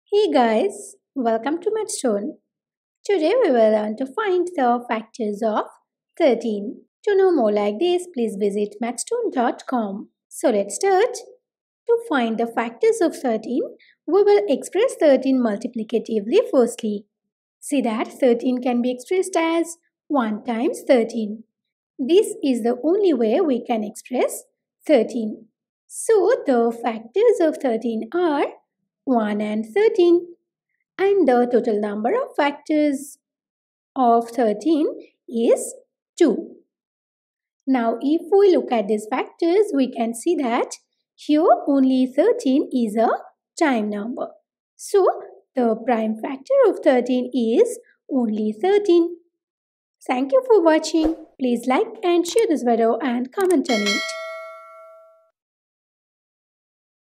Factors of 13 || Prime Factors of 13
Topic: What are the factors of 13?

In this video, we will find the prime factors of 13 in a very easy way.

We post videos daily on youtube